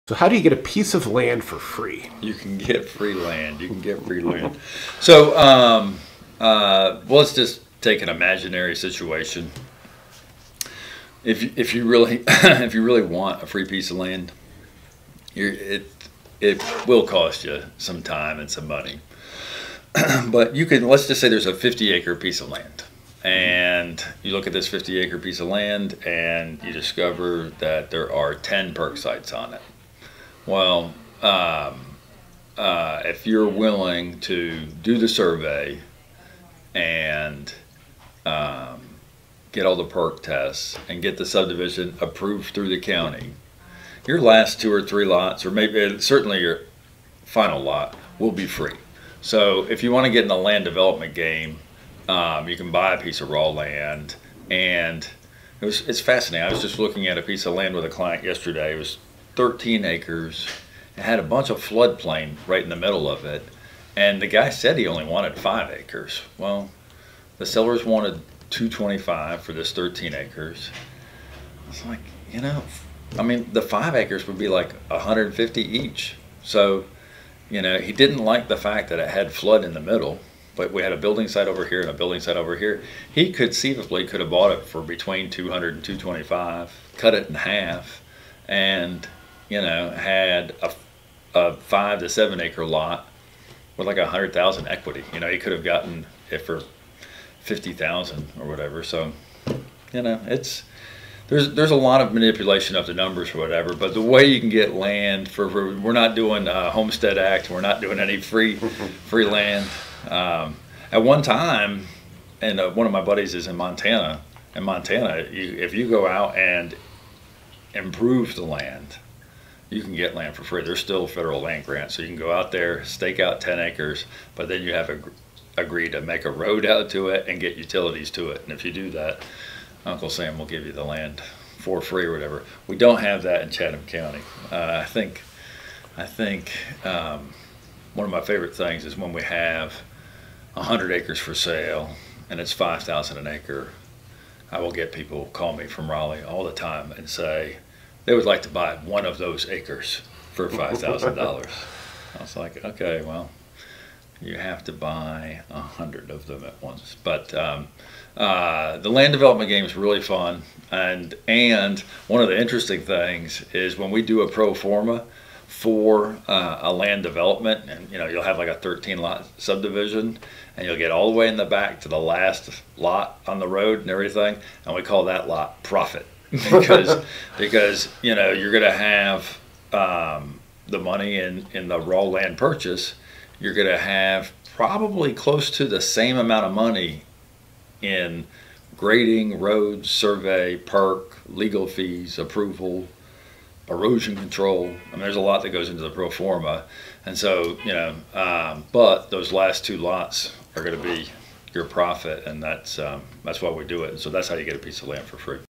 Land Hack - How To Earn Free Land in North Carolina
Land Expert Eric Andrews explains how to get cheap or free land in North Carolina. Hint: It does require some up front cost and investing.